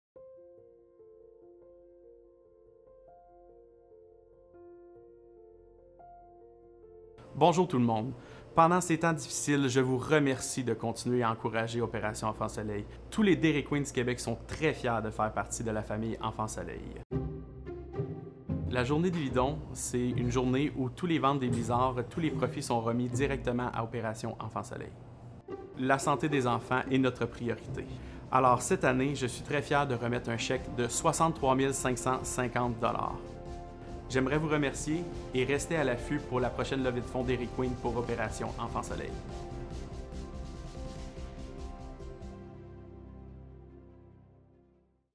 Dairy Queen OES
Promotion Dairy Queen pour ramasser des fonds pour Opération enfant soleil avec la journée annuelle Déli-don dont tout les profits des blizzards de cette journée sont remis.